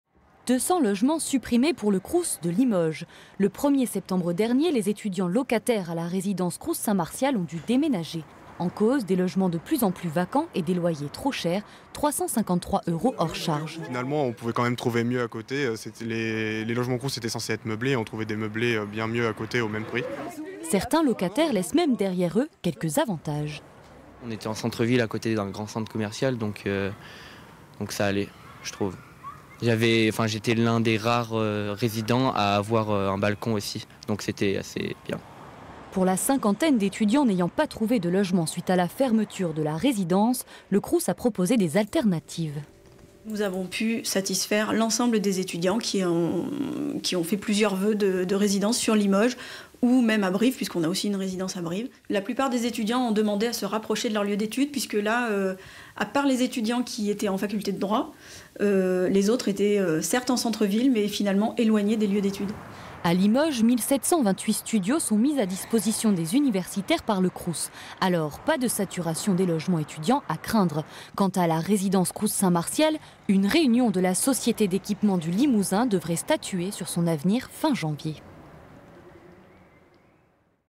200 logements étudiants en moins pour le Crous de Limoges
Le Crous de Limoges a décidé de ne pas renouveler le bail de la résidence Saint-Martial dont il était locataire rue Eugène Jamot. 200 logements étudiants ont été supprimés.

Journalistes : Colyne Rongere et Eric Boutin
Monteur : Nicolas Stil

Interview : Léo Bouladoux et Ethan Postic, étudiants et anciens locataires de la résidence Crous St Martial et Céline Baah, directrice du Crous de Limoges